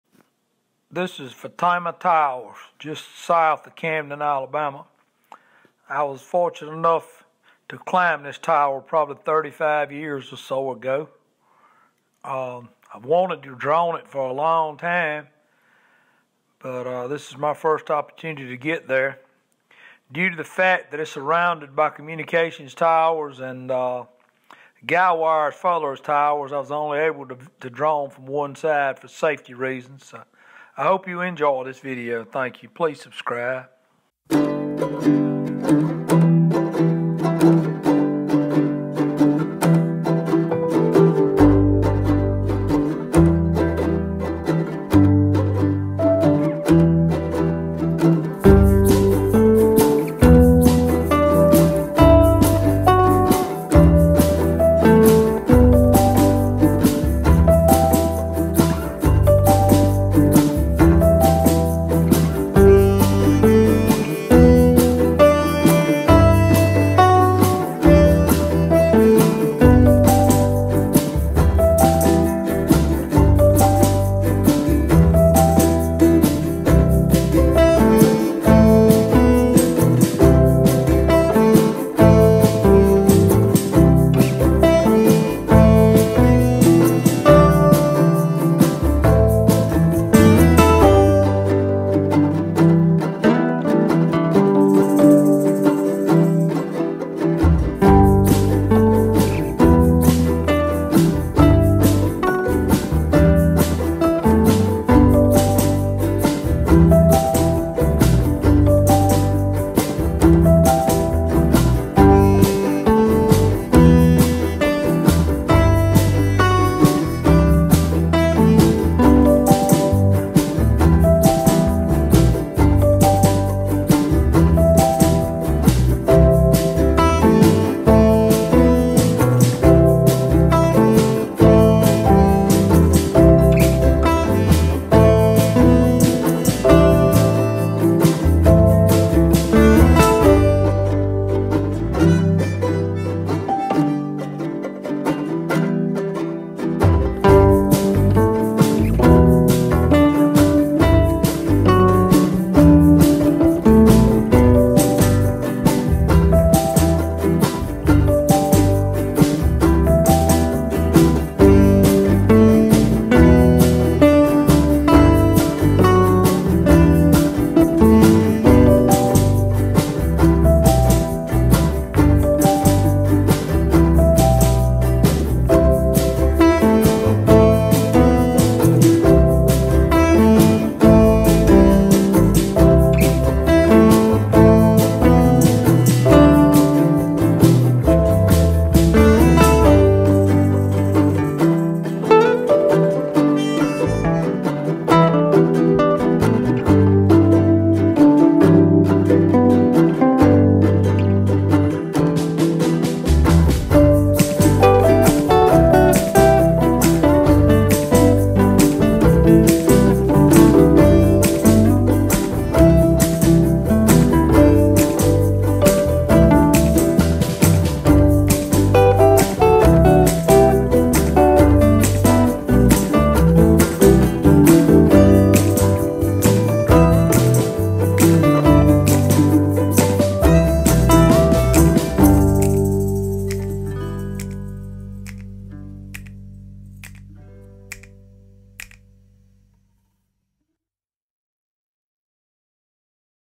Fatama Tower
Tower is located in Southern Wilcox County al. South of Camden just off of Al 265